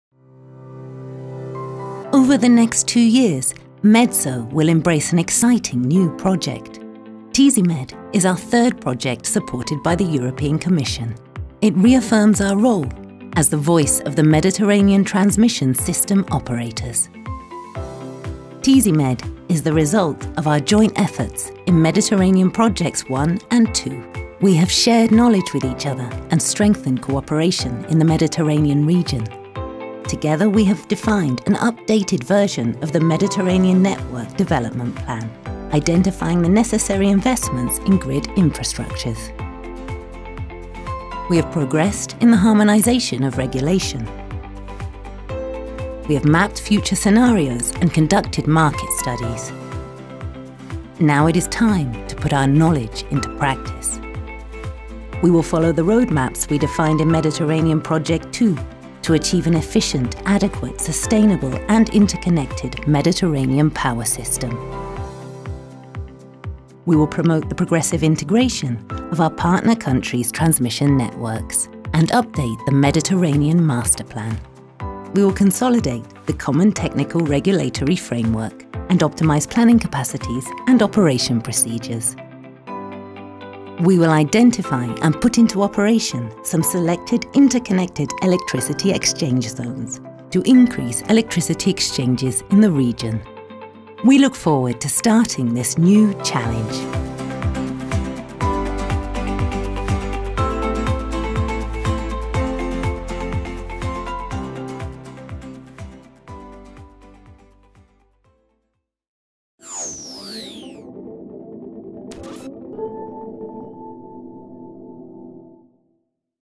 TEASIMED - a resilient power grid to facilitate the energy transition in the Mediterranean
Over the next 2 years, the Association of the Mediterranean Transmission System Operators (Med-TSO) will embrace a new exciting EC supported project called TEASIMED. This endeavour is the result of their joint efforts in Mediterranean Projects 1 and 2.

The knowledge shared by Med-TSO members will be now put into practice to achieve an Efficient, Adequate, Sustainable and Interconnected MEDiterranean power system.